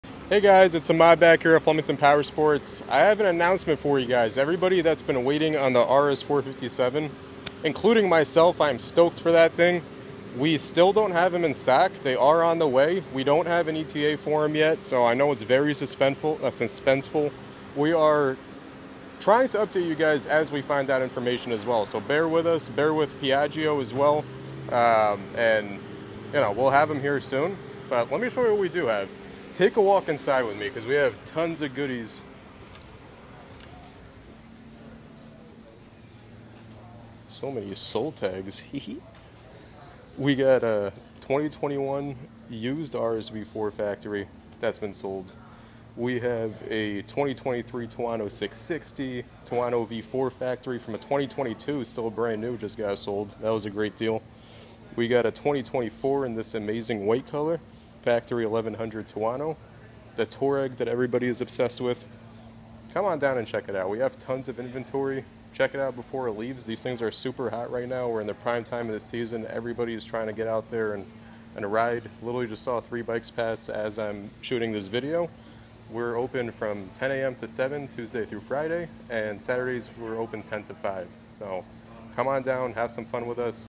Aprilia RS457 is coming! At Flemington Powersports
IMPORTANT NEWS!

Hey everyone! Exciting news from Flemington Power Sports - the highly anticipated @aprilia_americas RS-457 is on its way!! We expect the RS457 on either this Friday the 12th OR Wednesday the 17th! We’ll keep you updated. Hang in there, it’ll be worth the wait!